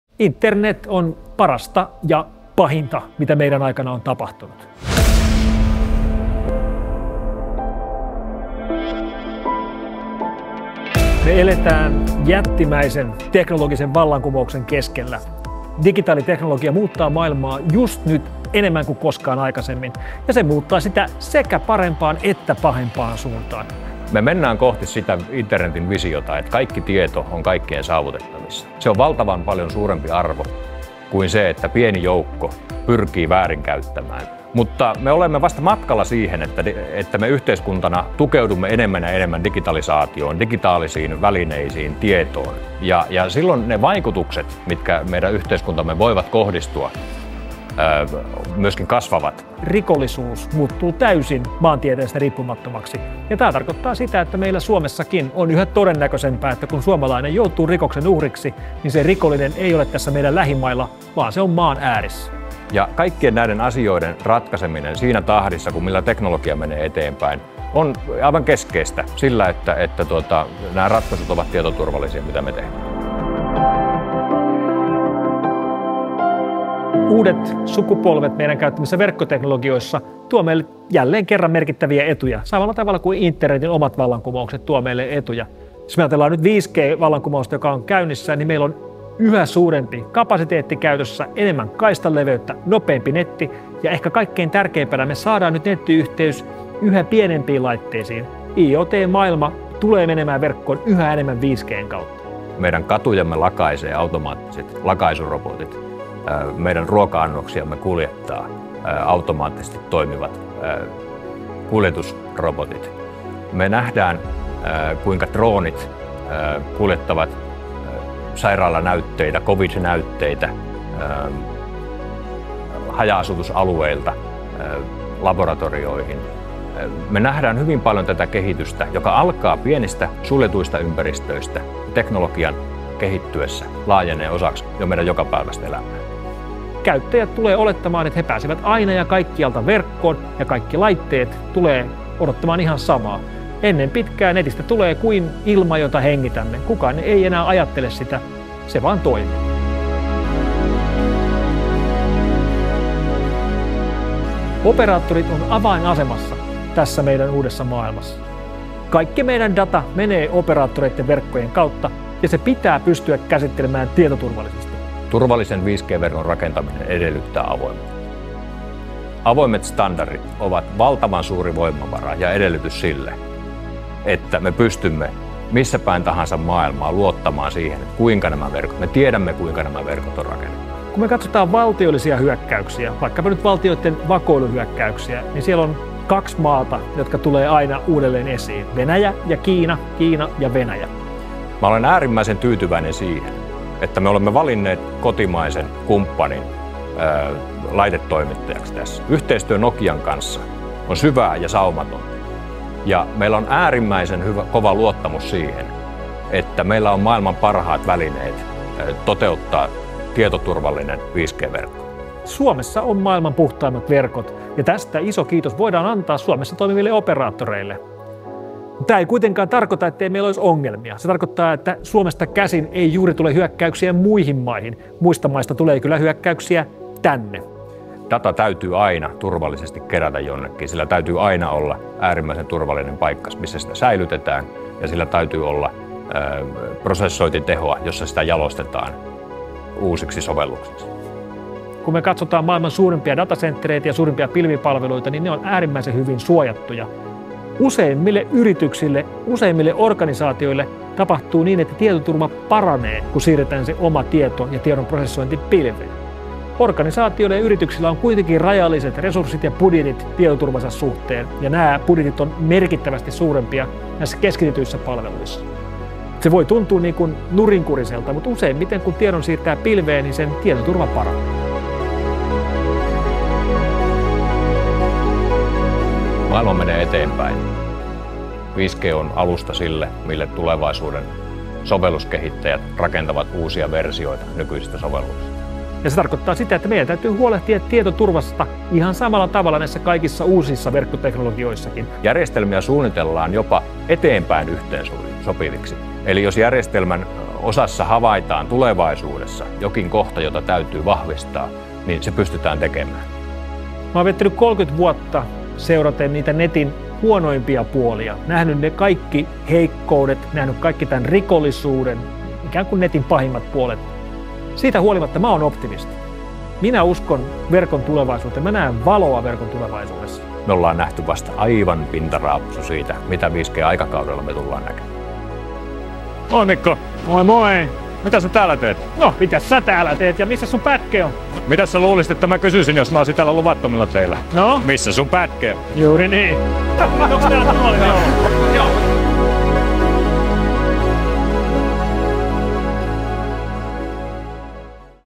Hyvä, paha internet
Telian 5G-johtaja Janne Koistinen kohtaa F-Securen tutkimusjohtaja Mikko Hyppösen Helsingin maaperän uumenissa.